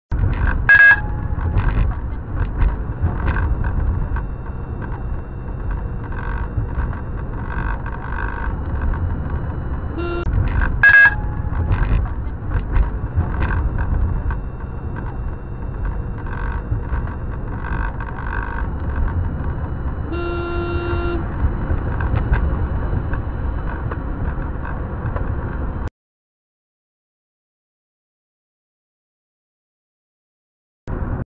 위험한 앞지르기 / 블랙박스TV
12월27일 중앙고속도로 의성ic 진입중 cj대한통운 택배차량의 위험천만한 앞지르기입니다.
차안에는 아내와 3살 아기가 타고있었는데..가슴이 내려 앉는줄 알았습니다.